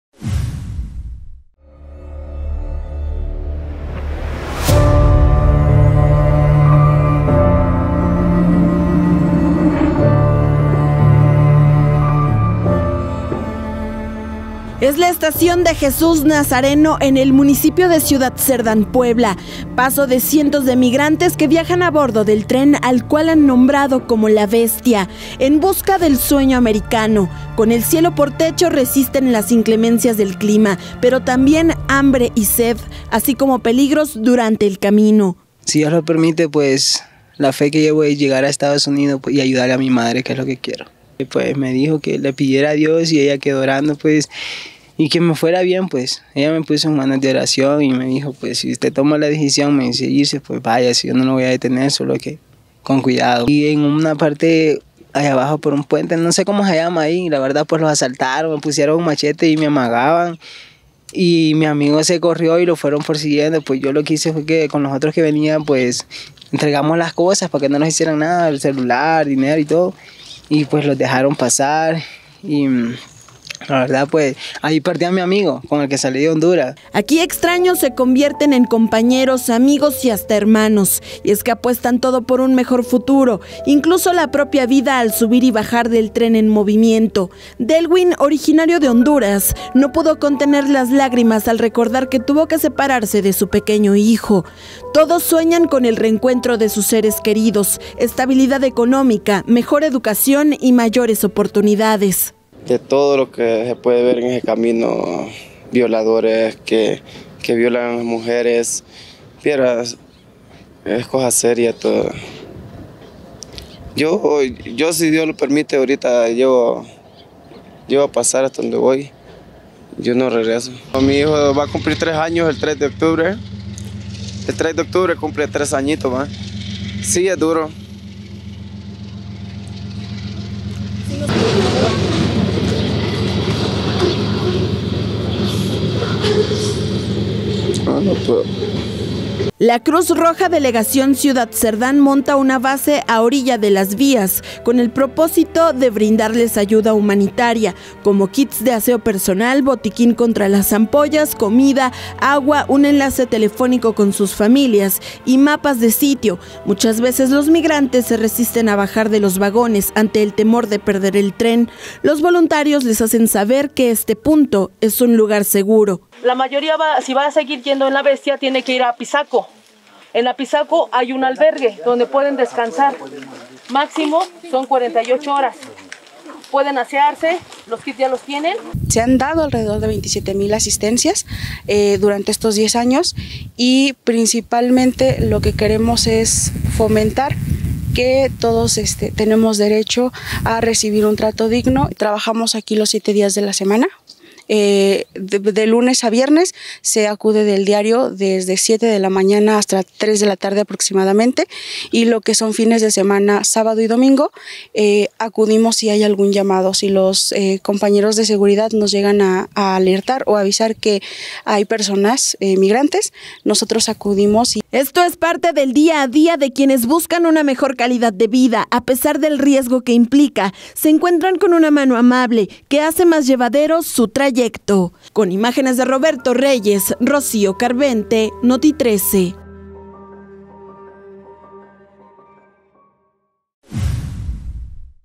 🔴 Brinda la Cruz Roja, apoyo para los migrantes que llegan a la estación de Jesús de Nazareno.
🔴 “Nos asaltaron con machetes… perdí a mi amigo con el que salí de Honduras”. En una carrera sin descanso, los migrantes lloran, sufren las inclemencias del tiempo y la delincuencia de México… pero en la estación de Jesús de Nazareno, la Cruz Roja, les da un respiro.